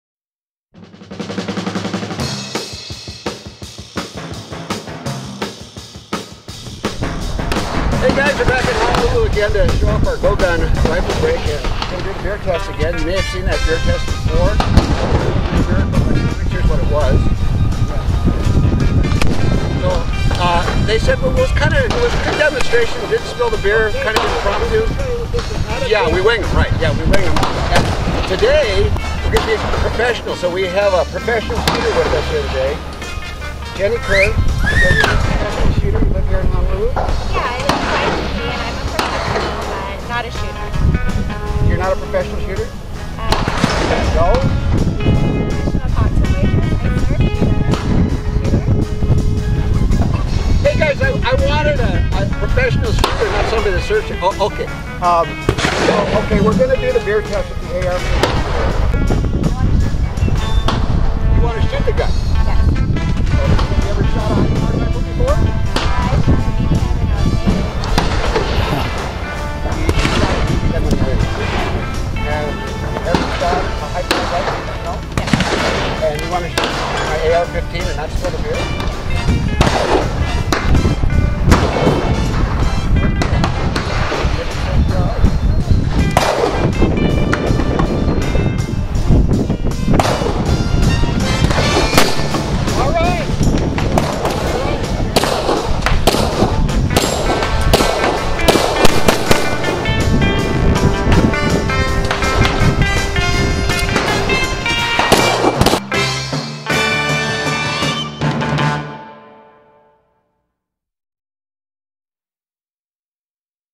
Jenny Does AR15 Beer Recoil Test, GoGun Talon Rifle Brake
Unbelievabl;e but true Jenny shoots AR 15 without spilling Beer, GoGun Talon Rifle Brake Rocks